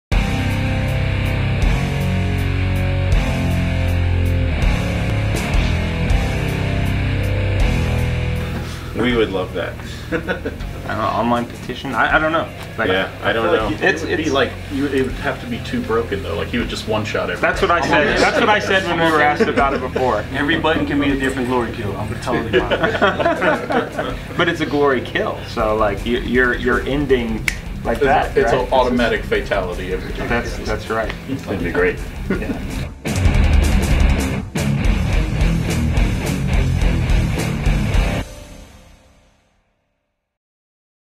QuakeCon 2019 Marty & Hugo Interview Part 14
In part 14 of 15 of our Marty and Hugo interview at QuakeCon, we talked about about getting the Doom Slayer in Mortal Kombat 11.